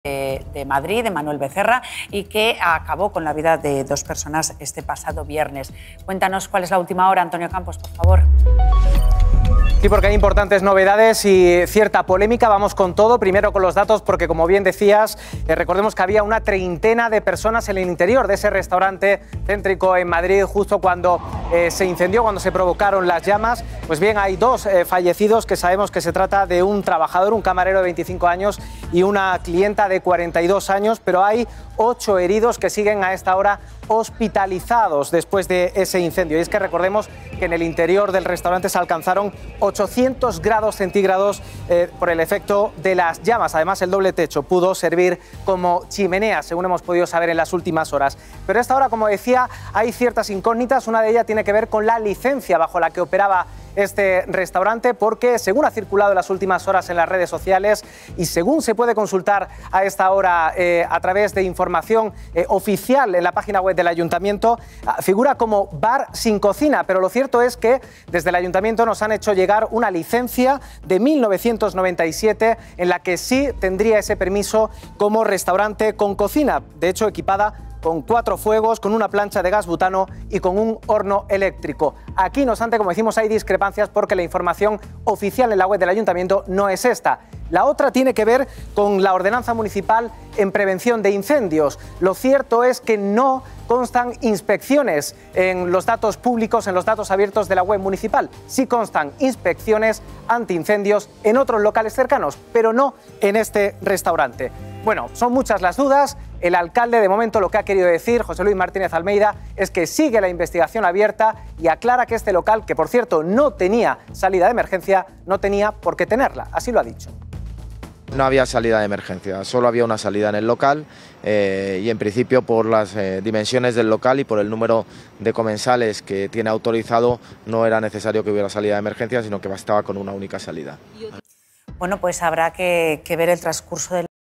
Última hora sobre el incendio en un restaurante italiano en Madrid, en ‘La Roca’
2 personas han fallecido y otras 8 han resultado heridas tras el incendio en un restaurante italiano en el centro de Madrid. Son un camarero y una clienta. El fuego se originó al flambear una pizza. El local quedó completamente calcinado.

Última hora en directo desde el plató de ‘La Roca’ el domingo 23 de abril de 2023.